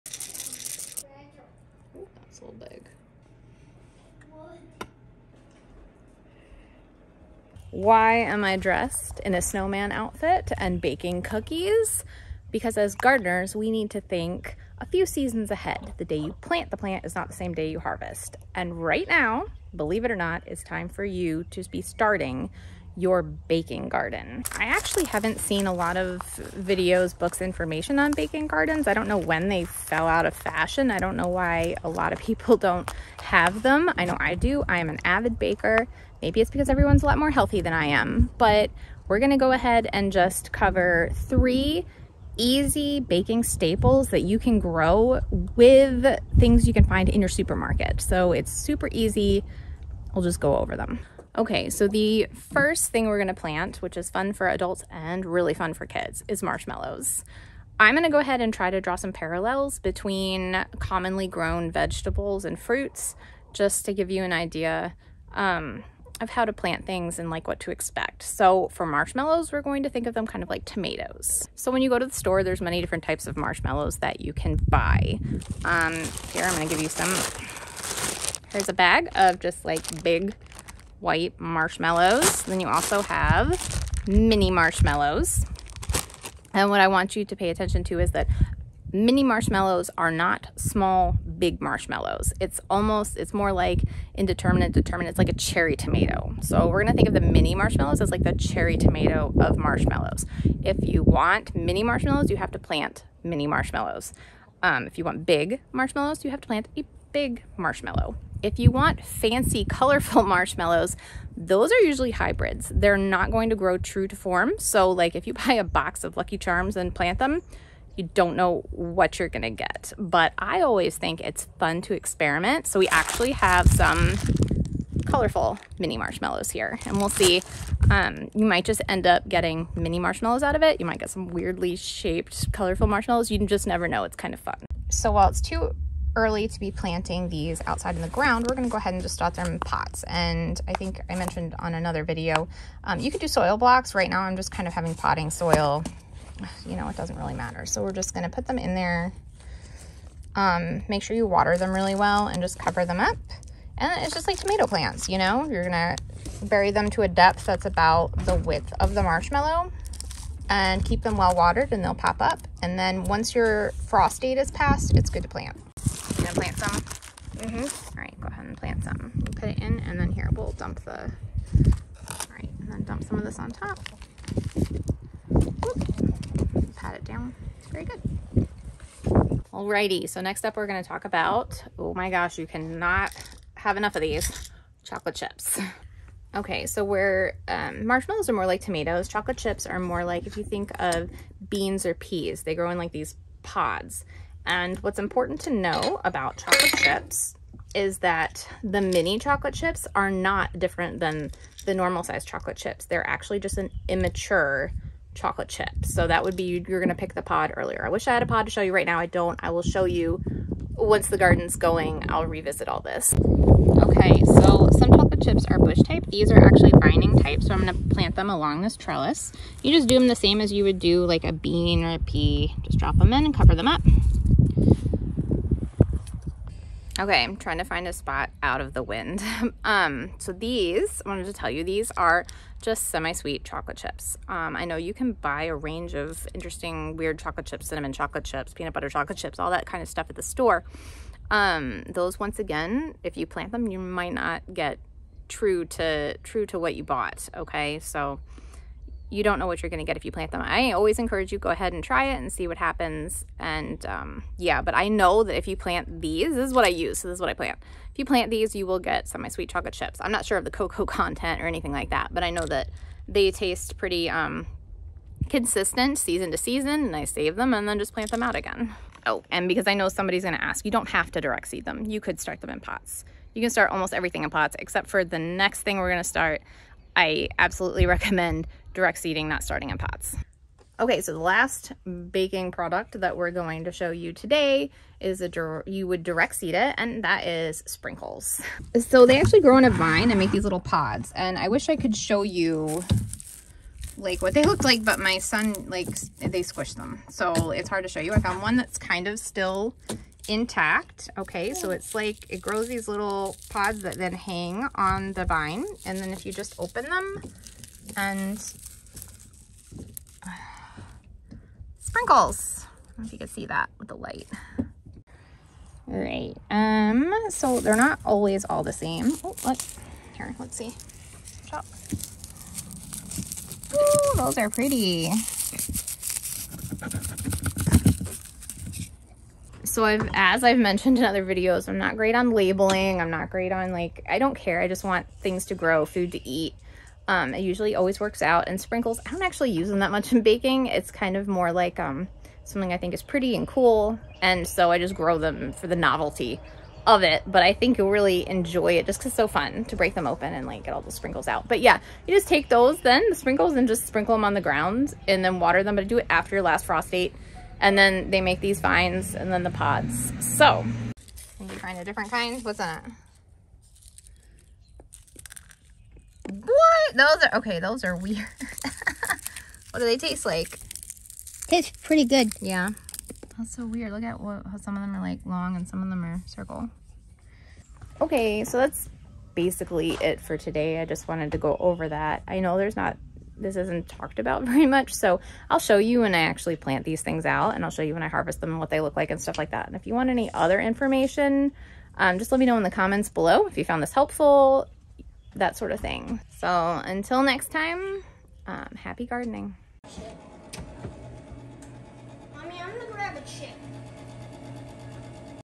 Planting an Old Fashioned Baking Garden | Homestead Garden How-to
It's time to start your Baking Garden! I haven't seen too many instructional videos on Traditional Baking Gardens, so I'm covering three baking staple plants that you can grow from the supermarket. Seed starting how to! Please leave questions below. Do you grow a baking garden?

Homestead Gardening